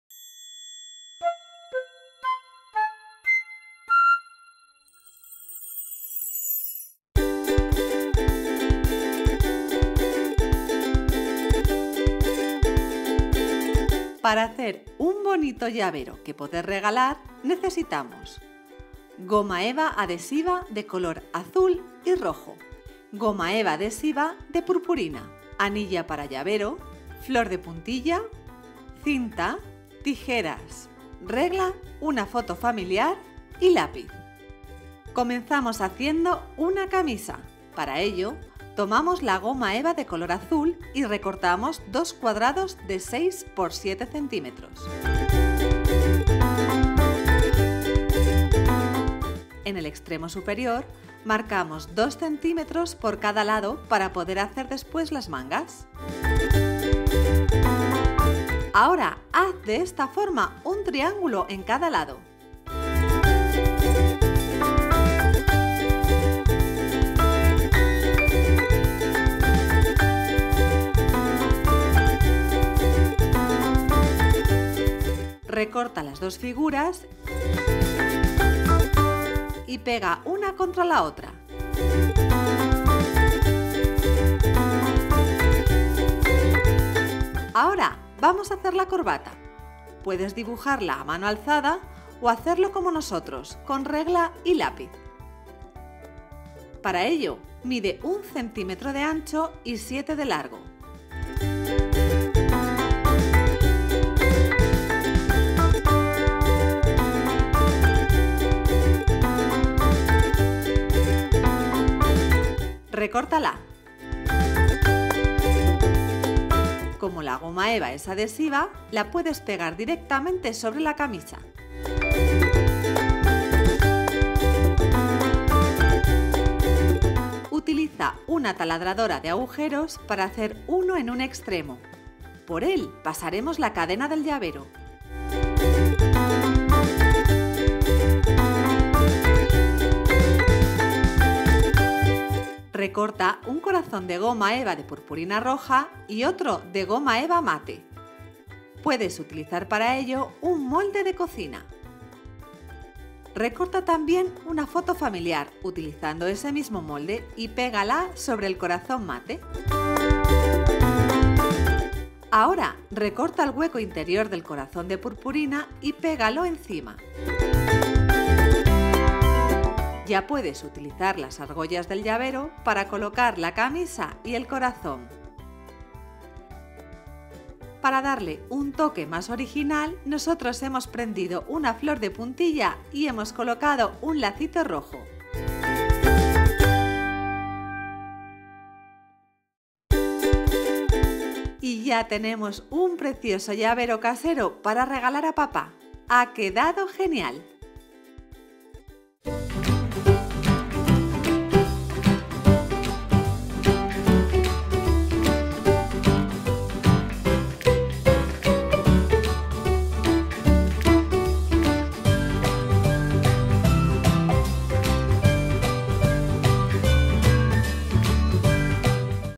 Llavero casero para regalar a papá | Manualidades del Día del Padre
¿Buscas un regalo casero bonito y original para el Día del Padre? En Guiainfantil.com te enseñamos cómo crear un llavero casero utilizando goma eva. Un detalle para papá que pueden elaborar los niños. ¡Feliz Día del Padre!

- Lista de materiales (00:18)
- Elaboración de la manualidad paso a paso (00:34)

En el siguiente enlace te mostramos cómo ahcer una tarjeta de plastilina para el Día del Padre para niños de 3 a 5 años:

En Guiainfantil.com podrás leer un cuento infantil para reflexionar sobre el papel de los padres: